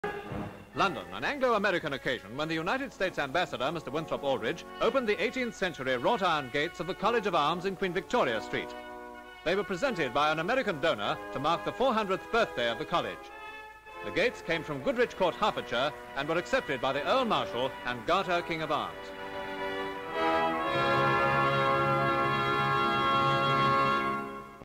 UK: U.S. AMBASSADOR OPENS NEW GATES FOR COLLEGE OF ARMS (1956)
NEW GATES FOR COLLEGE OF ARMS:

Full Description:

ALDRICH Mr WINTHROP. (U.S.) Opens the 18th Century GATES (wrought-iron of the COLLEGE OF ARMS in Queen Victoria St. shot of hand-w key.

COLLEGES Mr Winthrop Aldrich opens wrought-iron GATES in Queen victoria St. to mark the 400th B/Bay of the COLLEGE OF ARMS

GATE, gateway, doors, doorways...GATE, posts, doors posts ...etc An Angloo American occasion when Mr. Winthrop Aldrich & opened 18th Century wrought-iron gates of the College of Arms in Queen Victoria Street to mark the 400th B/Day of College.

LONDON. Mr Winthrop Aldrich opened the 18th Century GATES (wrought-iron) ...shot of hand opening..to mark the 400th B/Day of the COLLEGE OF ARMS...1956

Buildings, Landmarks and Monuments
Background: NEW GATES FOR COLLEGE OF ARMS: